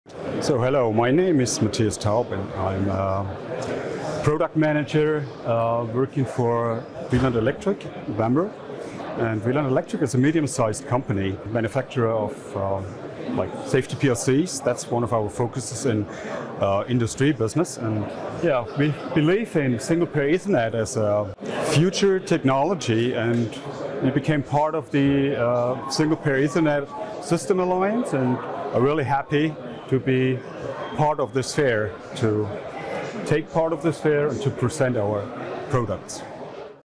Wieland Electric: The potential of Single Pair Ethernet
Matthias Taub (Wieland Electric) introducing his company and talking on the potential of Single Pair Ethernet for the industry.